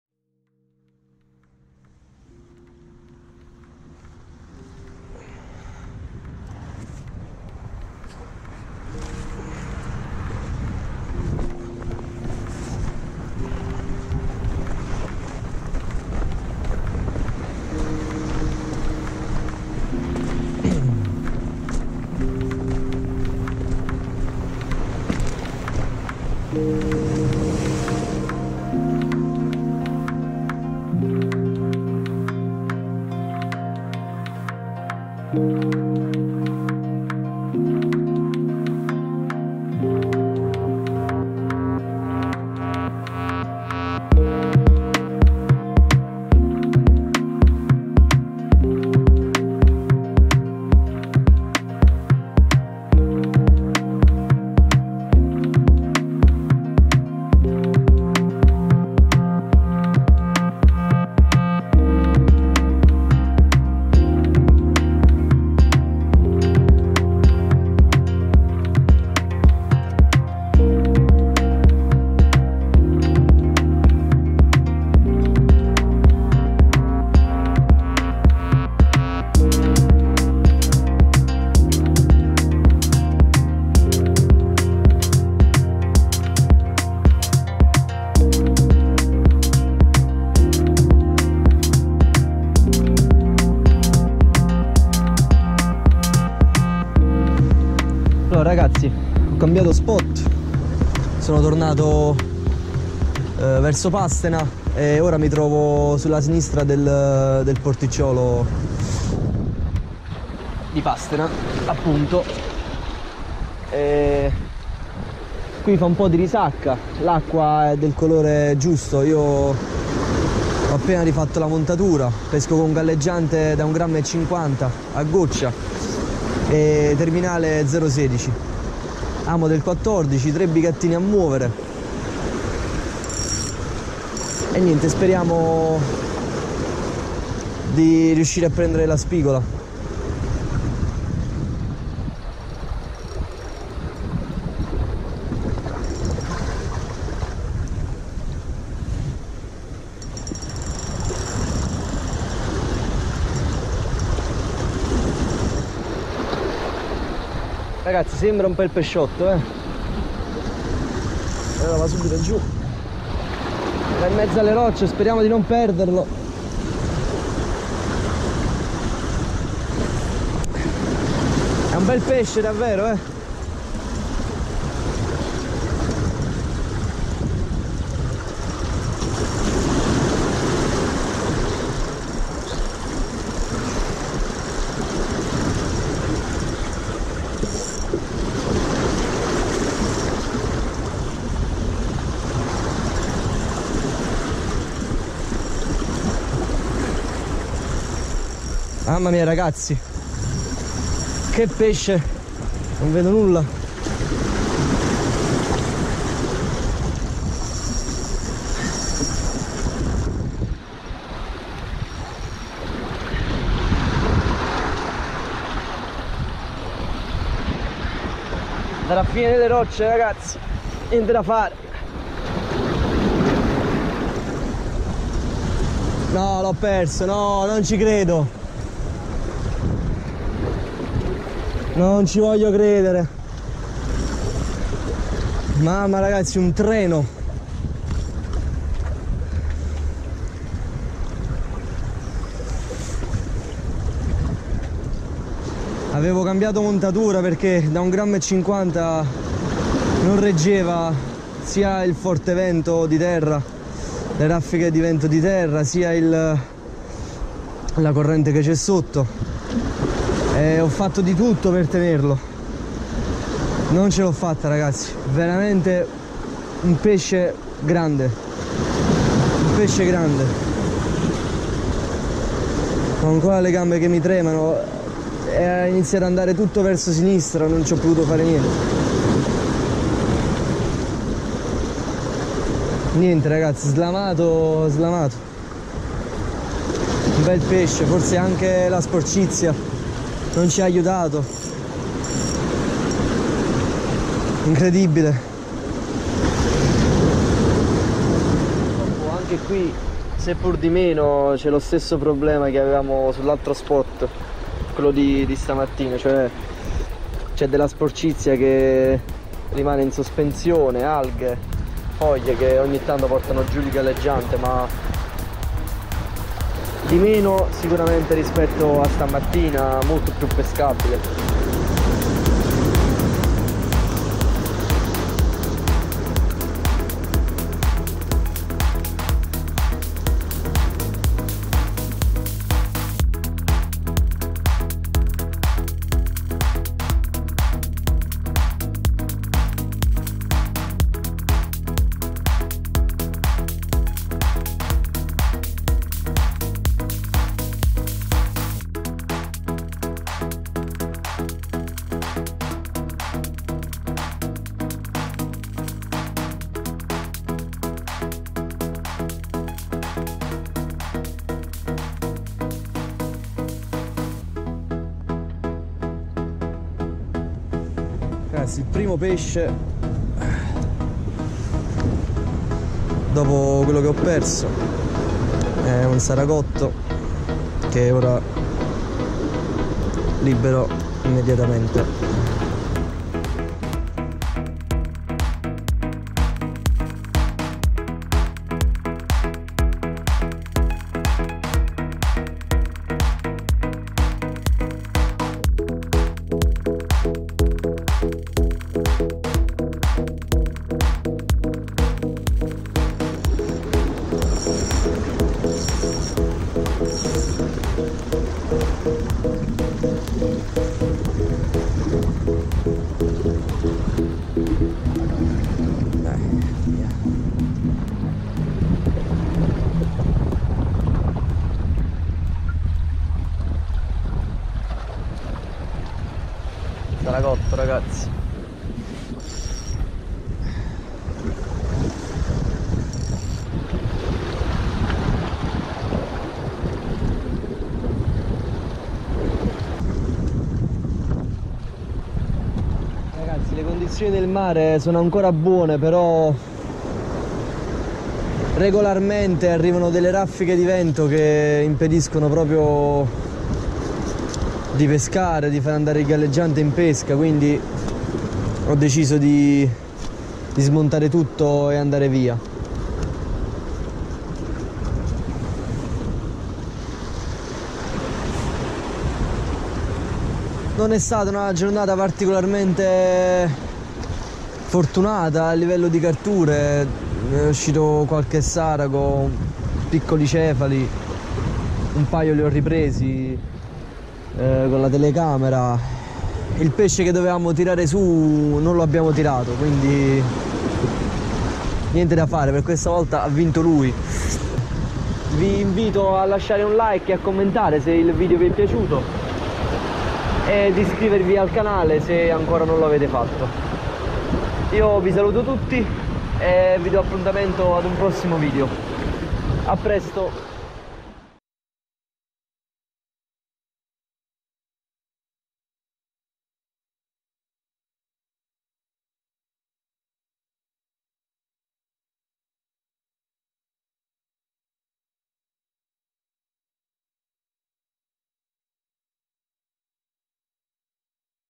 Pesca con la Bolognese | Mare in Scaduta
Dopo vari  spostamenti  riesco a trovare uno spot con meno sporcizia possibile. Riesco ad agganciare un bel pesce, forse il più grande  che mi sia capitato con questa tecnica :  quando effettivamente realizzo che in canna c'è del peso da non sottovalutare ... è adrenalina a mille! Saper gestire le emozioni in questi momenti è fondamentale.

Ciao a tutti e buona visione.